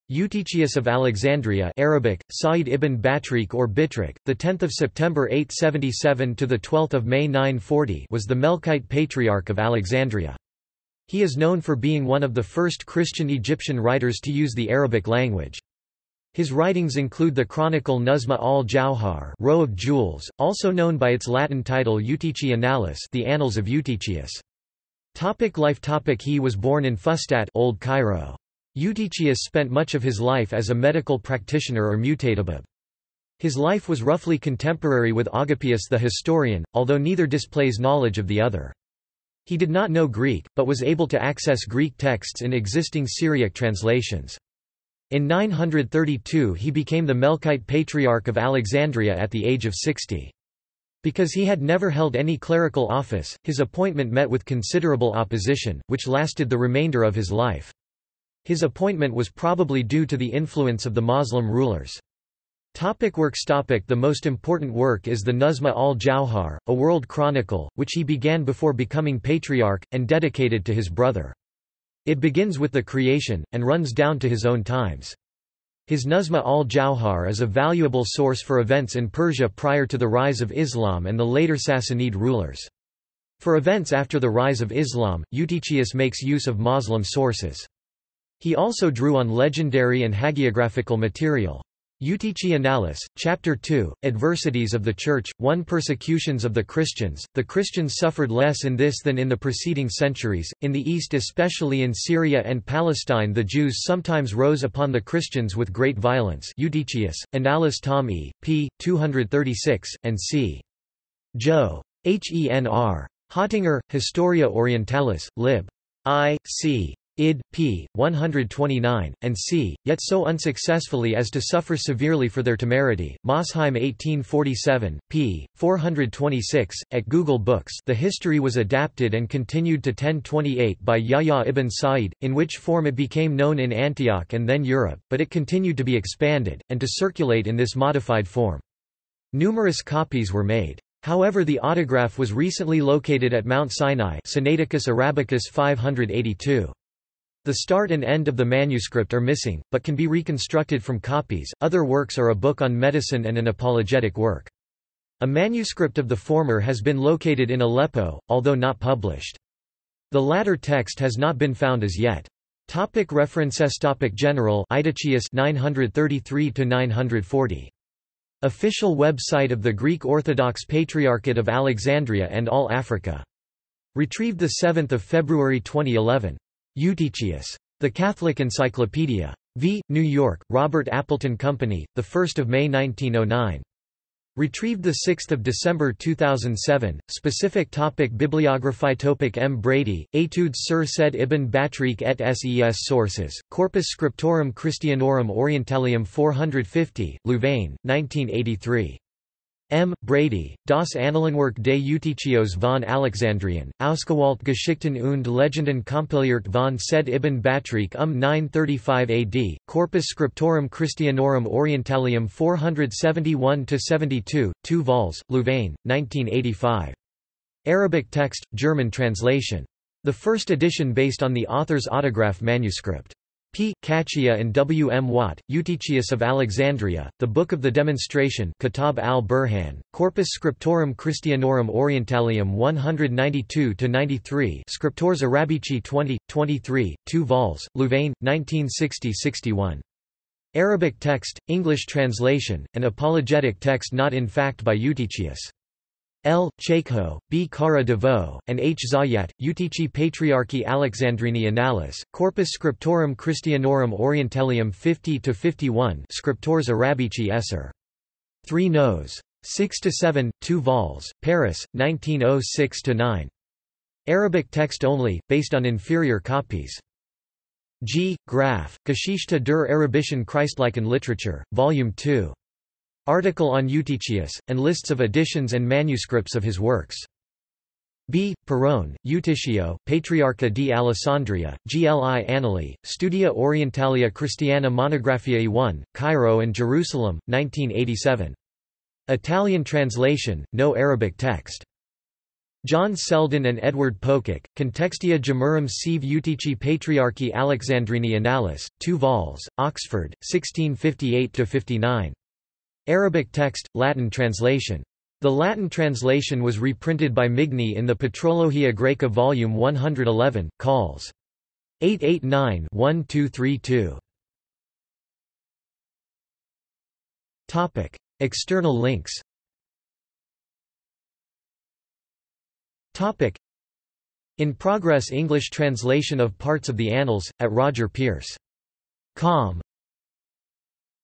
Patriarch Eutychius of Alexandria | Wikipedia audio article
This is an audio version of the Wikipedia Article:
Patriarch Eutychius of Alexandria

SUMMARY
Eutychius of Alexandria (Arabic: Sa'id ibn Batriq or Bitriq; 10 September 877 – 12 May 940) was the Melkite Patriarch of Alexandria. He is known for being one of the first Christian Egyptian writers to use the Arabic language. His writings include the chronicle Nazm al-Jauhar ("Row of Jewels"), also known by its Latin title Eutychii Annales ("The Annals of Eutychius").